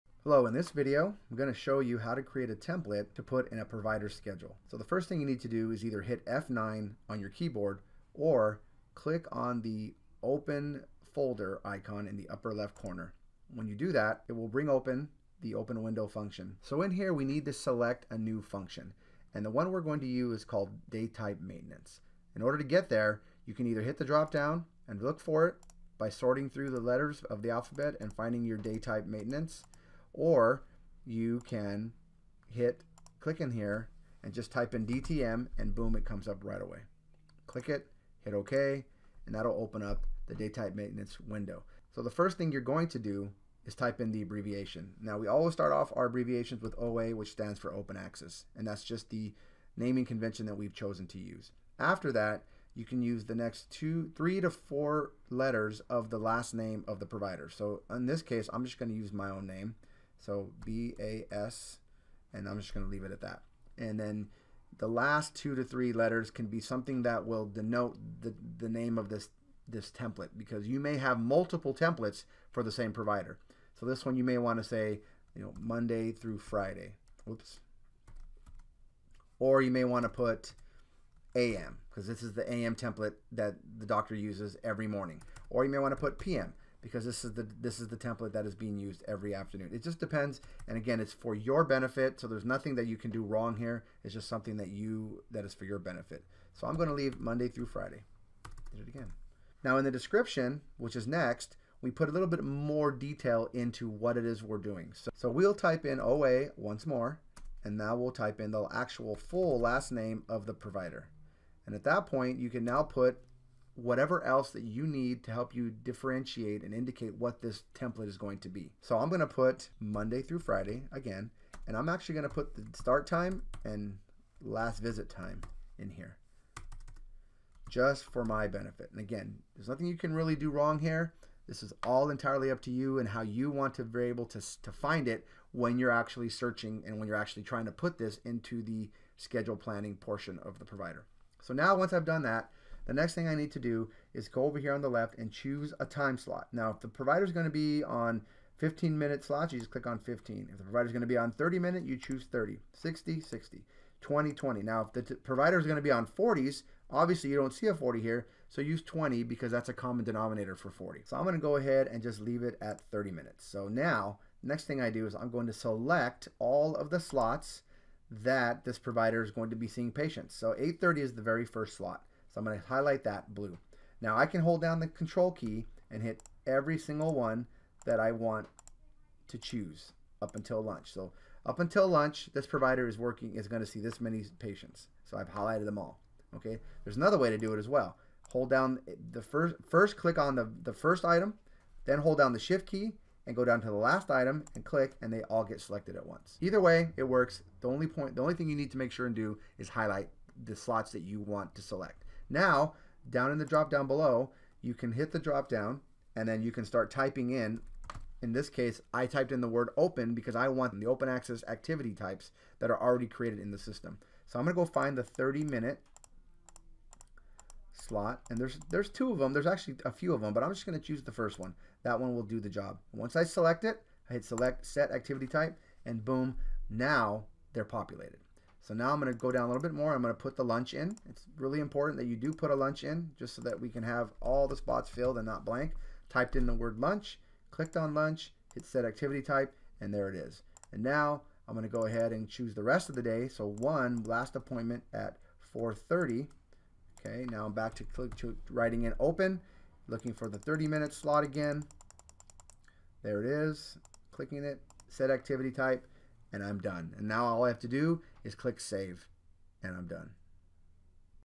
How to Create a Provider Schedule Template
Video instruction on how to create a template in the Practice Management (PM) of Allscripts Touchworks program